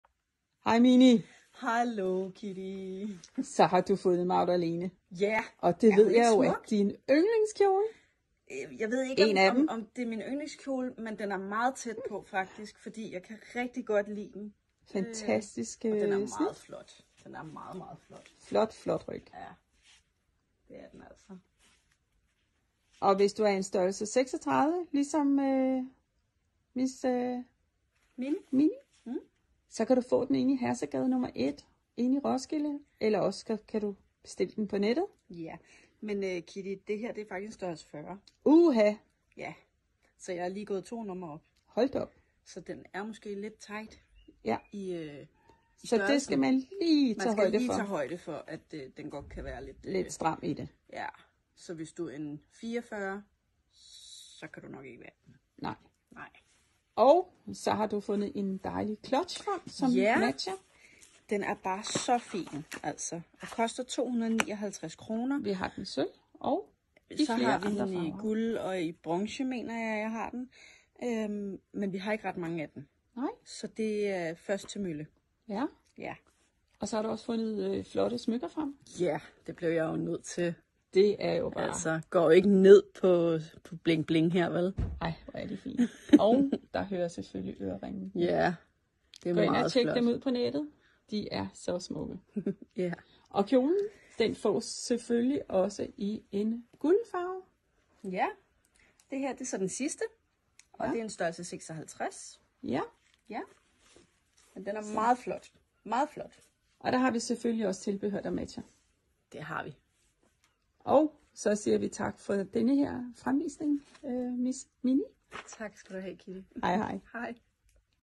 Lang gallakjole/brudepigekjoler - Magdalene - smuk chokoladefarvet/sølvfarvet festkjole havfruemodel
Lang gallakjole - Magdalene - smuk chokoladefarvet lang festkjole med blonde i havfruemodel

Materiale:
Shell1:40%Polyester 40%Nylon 20%Cotton
Shell2:100%Nylon
Lining:100%Polyester
Kjolen har en diskret lynlås i ryggen
Kjolen tåler vask i hånden eller rens

Du kan med fordel se på vores Liv Leonora blondebolero til denne kjole.